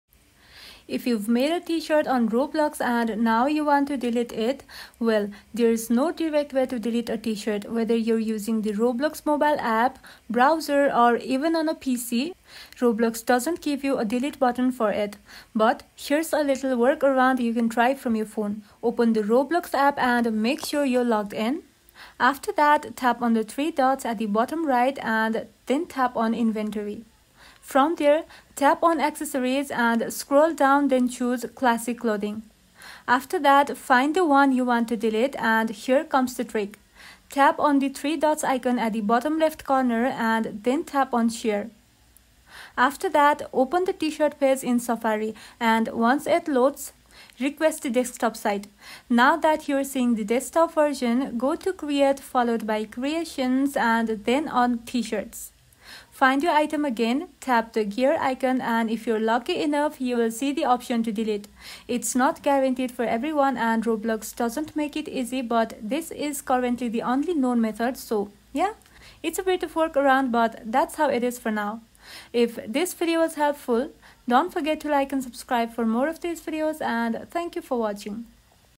How To Delete the T-Shirt You Made in Roblox (2025 Guide)
0:05 - Go to the Roblox website, click on “Create,” then select “Manage My Experiences” under the T-Shirts section
0:10 - Click the gear icon next to the T-Shirt you want to delete, then choose “Delete” to remove it permanently

Note: You can only delete T-Shirts you’ve uploaded; this won’t refund any Robux spent